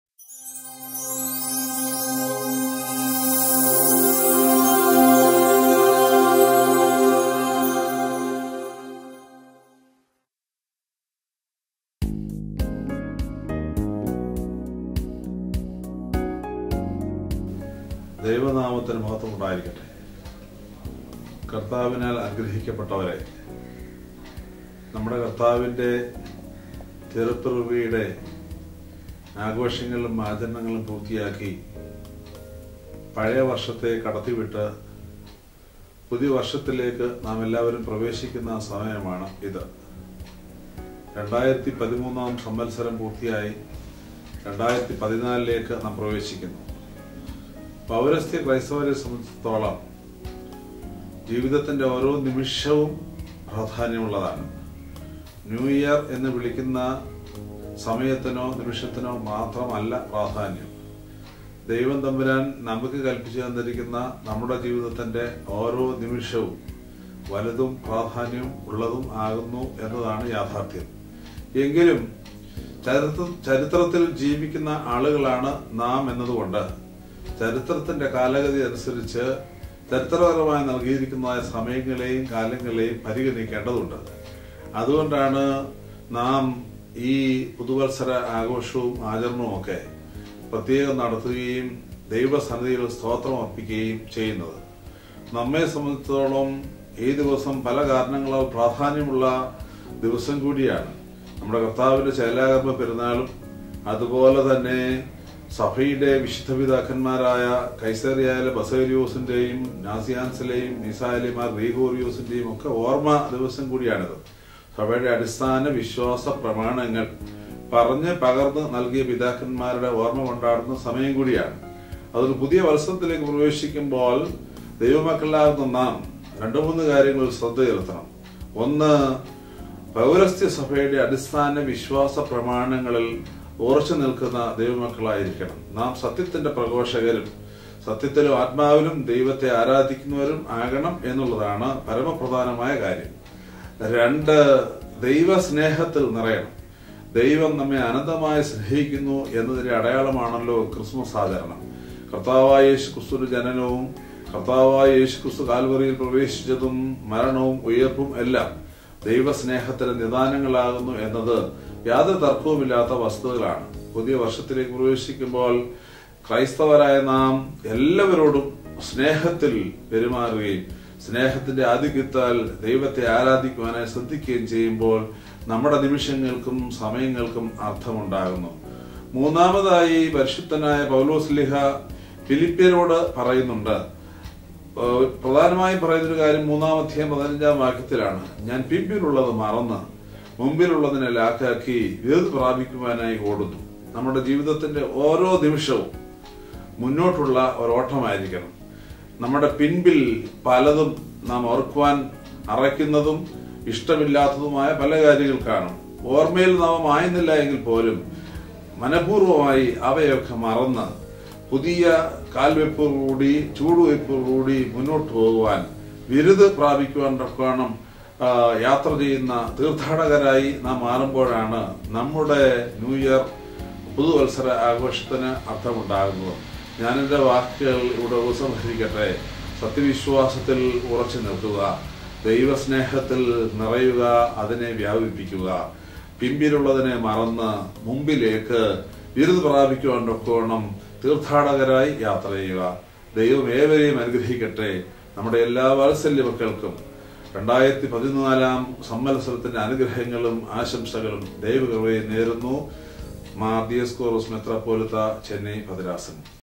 New Year Message by HG Dr. Yuhanon Mar Diascoros
New Year Message by HG Dr. Yuhanon Mar Diascoros (Metropolitan of Chennai Diocese)